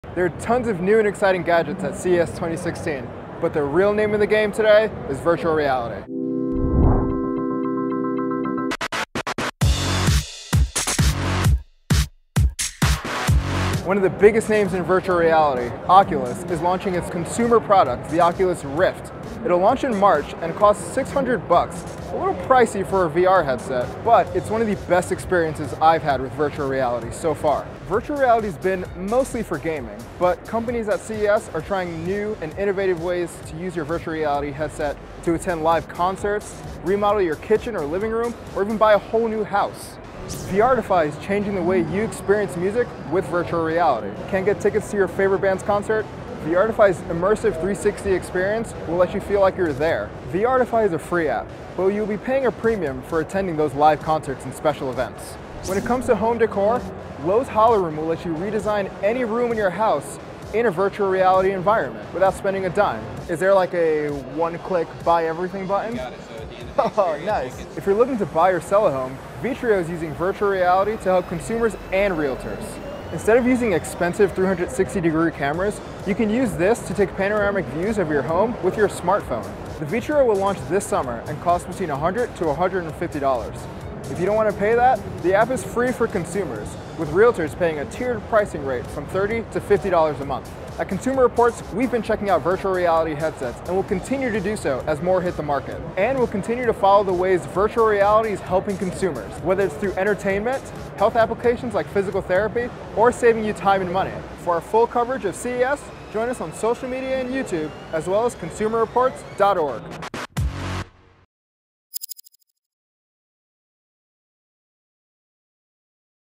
Virtual Reality Pushes Beyond Gaming | Consumer Reports
Tech companies are using virtual reality to allow people to do things like walk into their new kitchen before it's actually remodeled or experience a live concert from home as if they were there, all of which saves time and money.